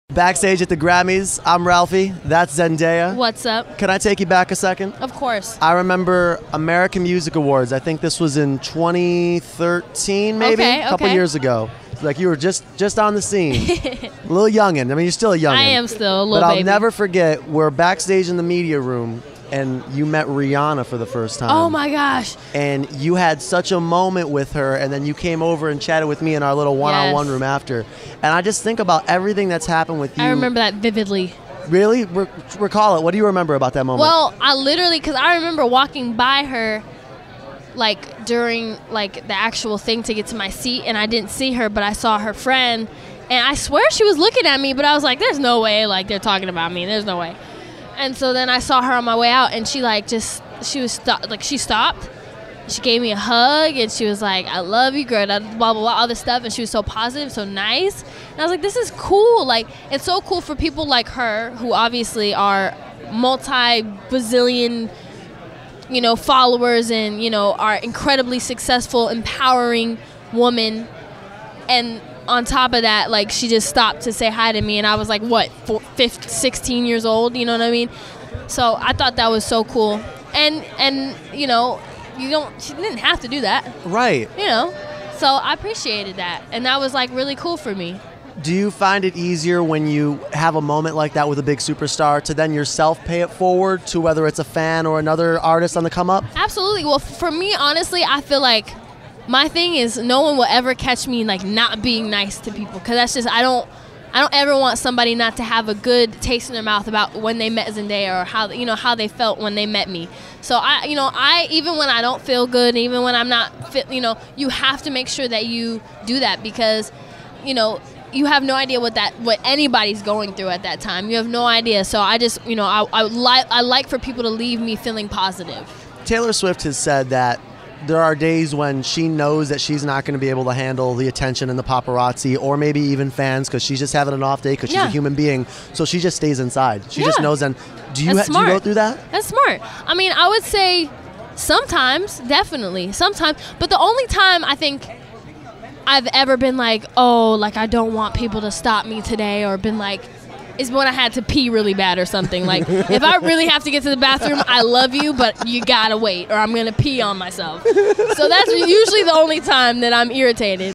The 58th GRAMMYs WWOBackstage: Zendaya Recalls Meeting Rihanna
Backstage at The 58th GRAMMYs with Westwood One, Zendaya talked about meeting Rihanna for the first time and her new song which features... Chris Brown.